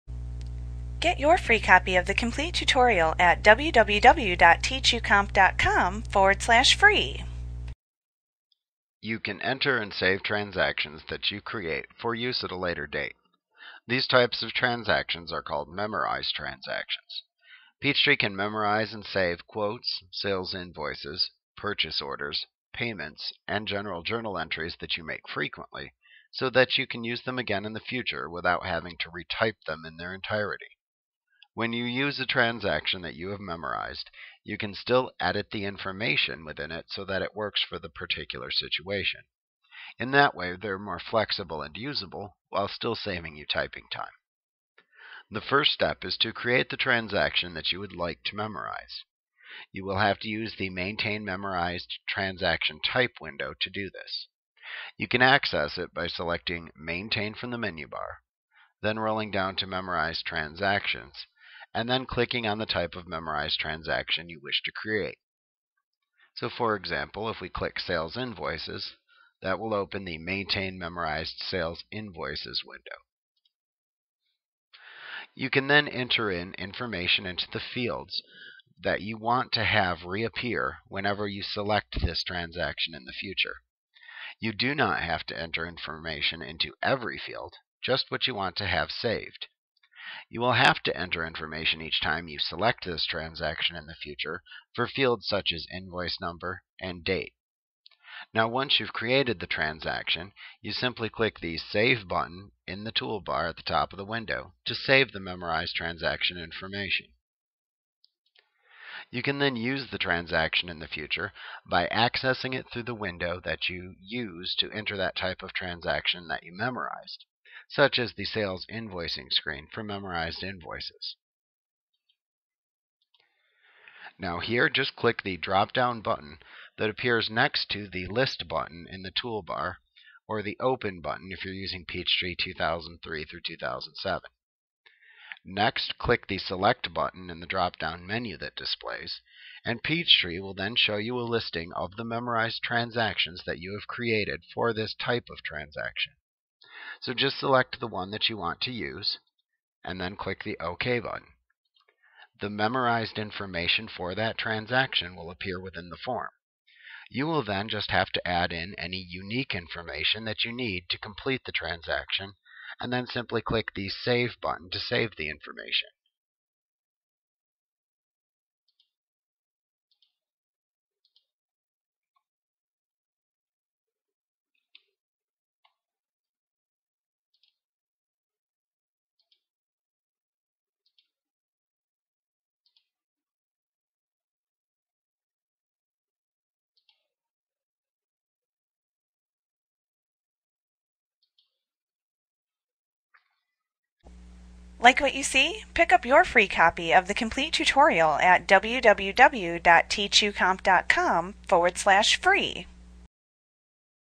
Peachtree Tutorial Memorized Transactions Sage Training Lesson 13.3
Learn about memorized transactions in Sage Peachtree A clip from Mastering Peachtree Made Easy v. 2011. - the most comprehensive Peachtree tutorial available. Visit us today!